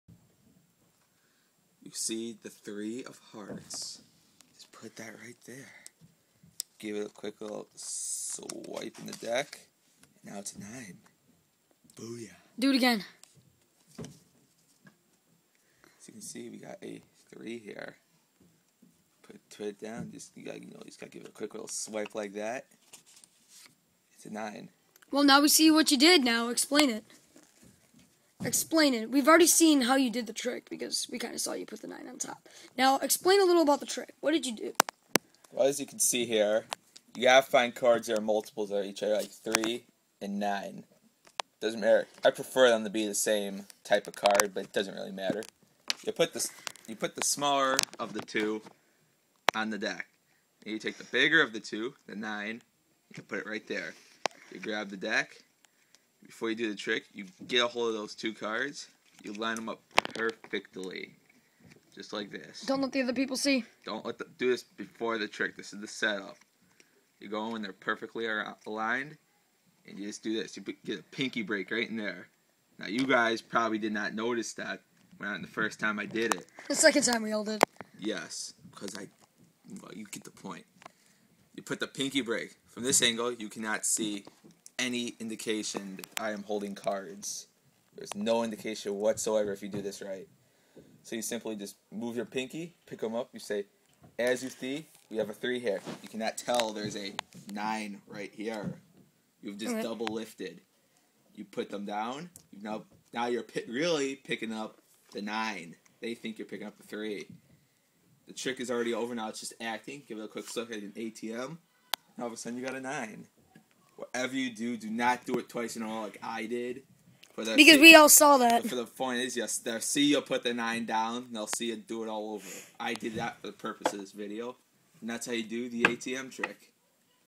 Alec Spade- ATM machine card trick explained
Alec Spade demonstrates the ATM machine trick